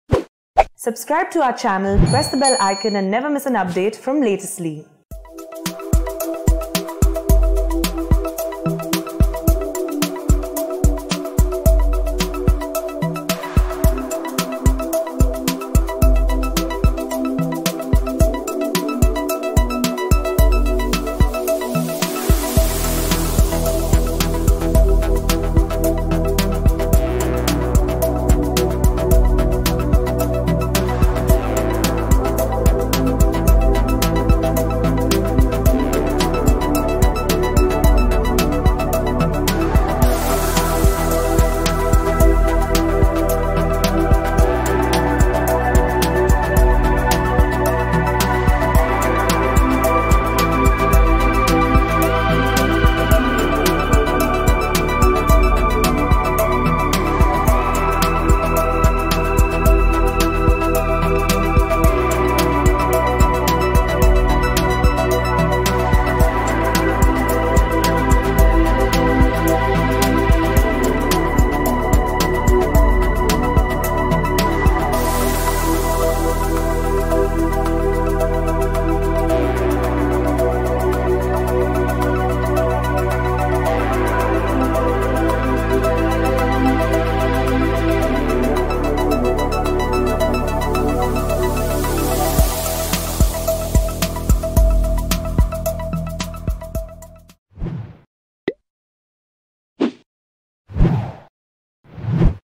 Vande Bharat Express Train: Stones Pelted Between Thirunavaya & Tirur In Kerala, Window Glass Broken
On May 1, some miscreants allegedly pelted stones at the Vande Bharat Express train. The incident took place when the recently launched train was travelling between Thirunavaya and Tirur. The stone pelting incident took place around 5 pm and Railway officials informed the police immediately. Police said the train continued its journey to Thiruvananthapuram. No injuries were reported. Watch the video to know more.